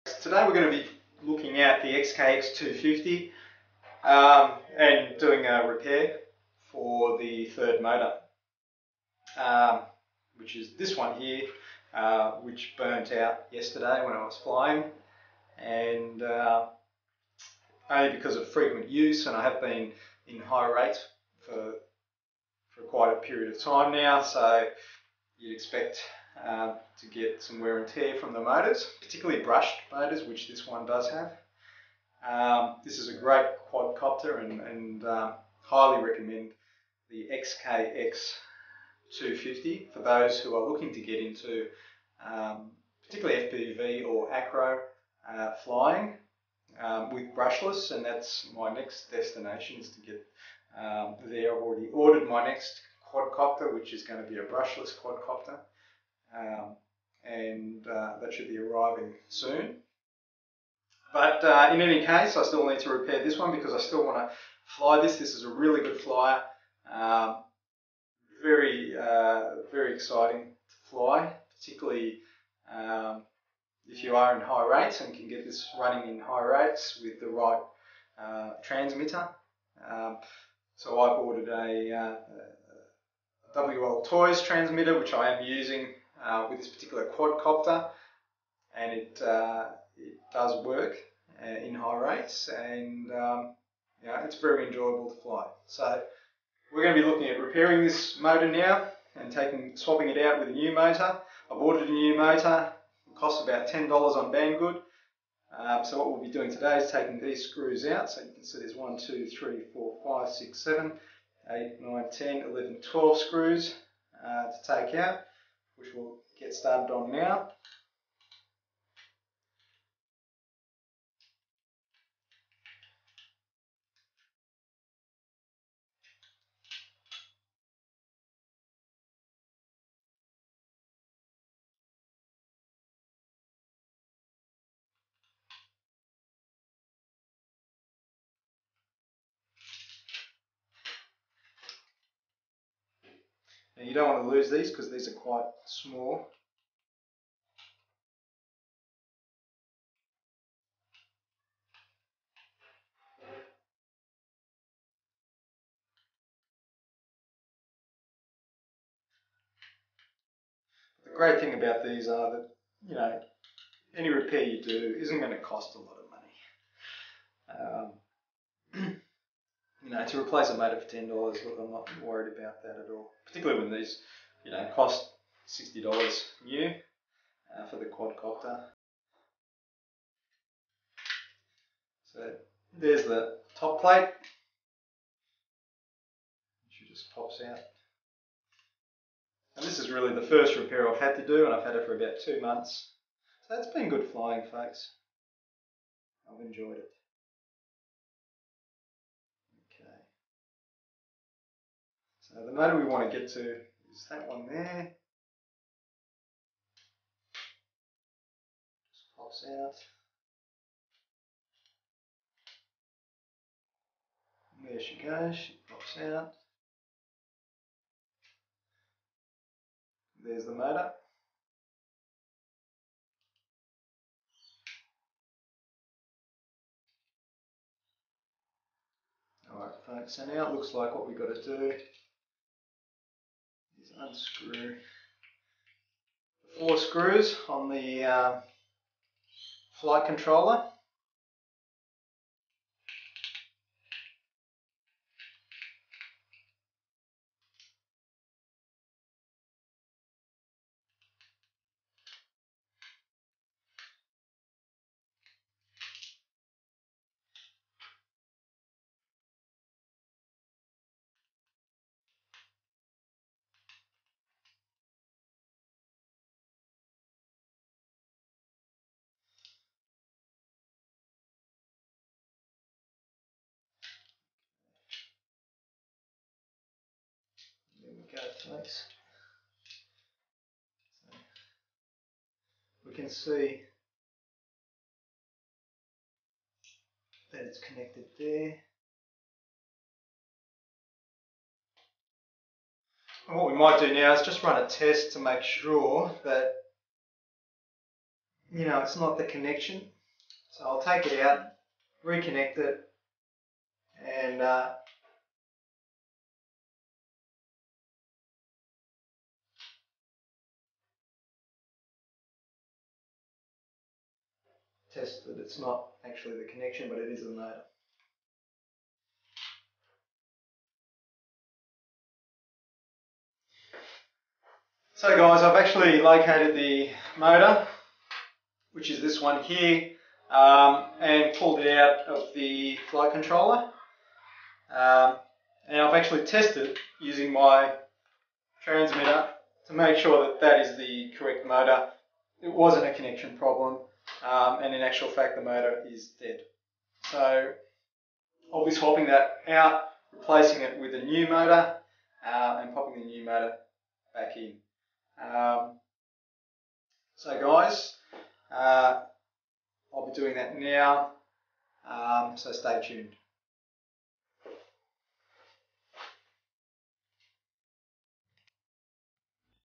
XK X250 Motor Repair Part 1
Replacing a brushed motor on my XK X251 Quadcopter - Part 1

The motor burnt out during flight, I noticed that the speed of the quad had reduced considerably and had no pitch. After landing I noticed that one of the motors was not spinning up when applying throttle. This is Part 1 of 2 where I take the motor out of the Quadcopter. Thanks!